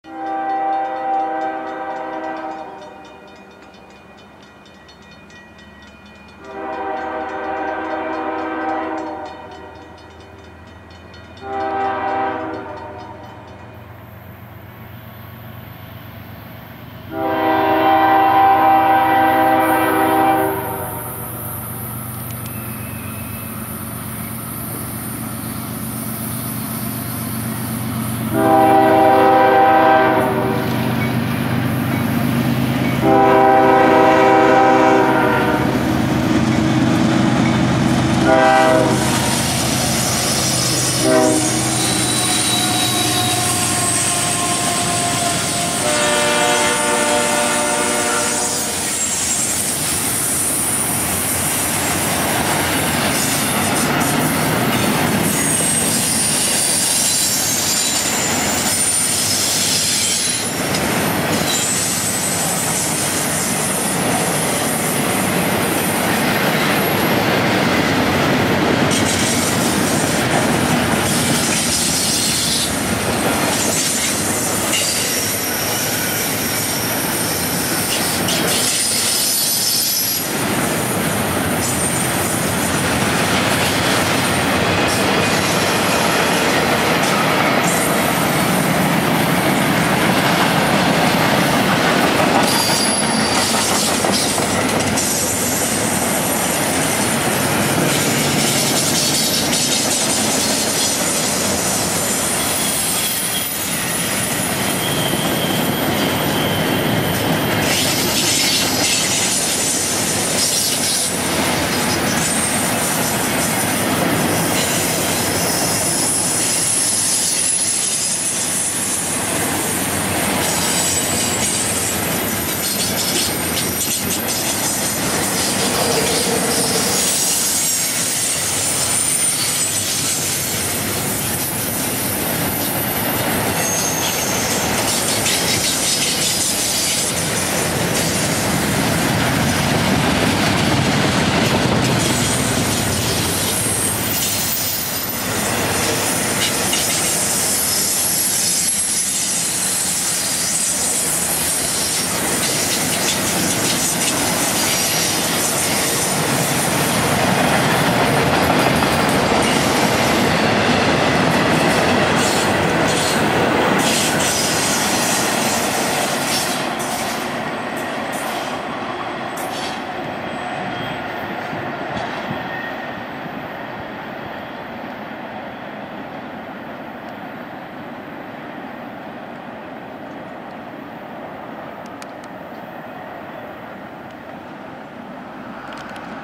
Southbound CSXT stack train in macon county.
CSX train, on a cold morning.

CSX: C44-9WAC #140

at the beaver creek, home of the duck race.

Montezuma, Ga.